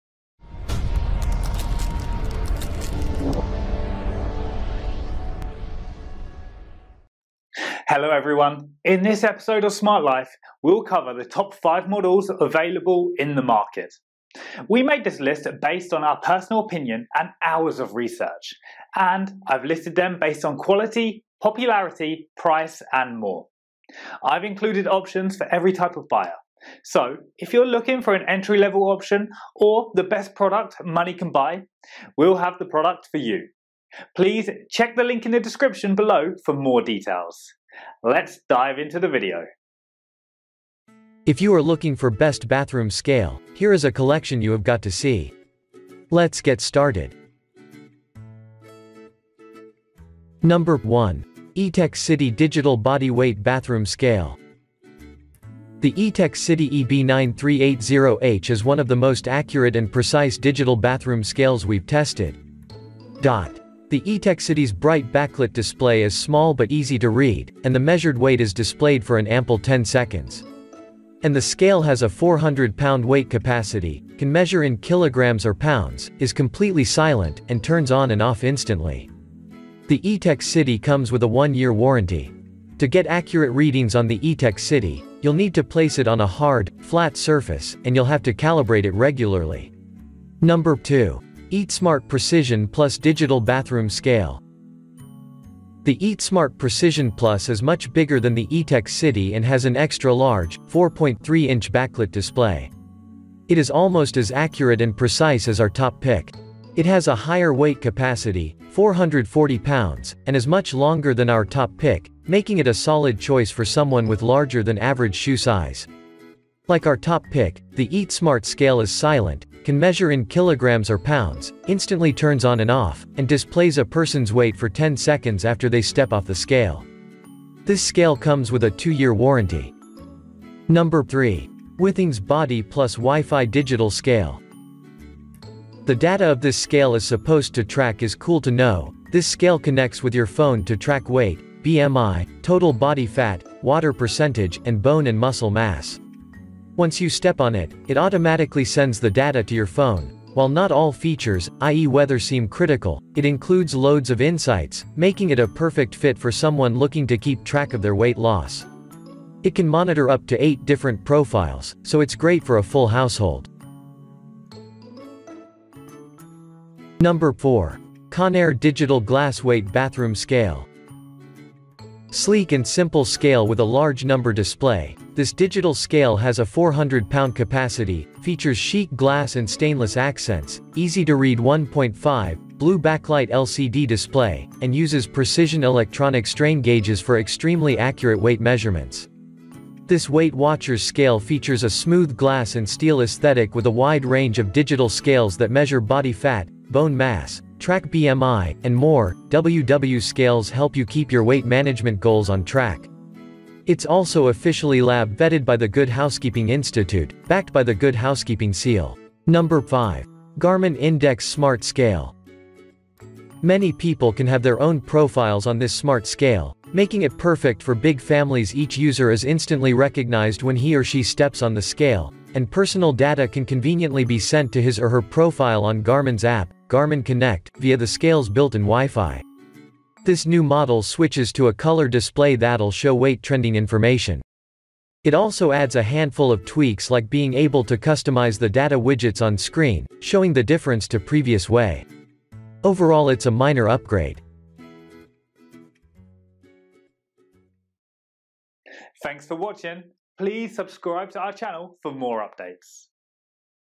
Top 5 Best Bathroom Scales Review in 2023
Here is the list of five best bathroom scales:
1. Etekcity Digital Bathroom Scale
2. EatSmart Precision Bathroom Scale
3. Withings BodyDigital Scale
4. Conair Digital Bathroom Scale
5. Garmin Index Smart Scale

🕕Timestamps🕕
0:42 - Introduction
0:51 - Etekcity Digital Bathroom Scale
1:30 - EatSmart Precision Bathroom Scale
2:10 - Withings BodyDigital Scale
2:50 - Conair Digital Bathroom Scale
3:30 - Garmin Index Smart Scale

🤨 What is a bathroom scale?
The term Bathroom Scale refers to a kind of protection industrial scales have protection against mud, water, dust, and numerous substances.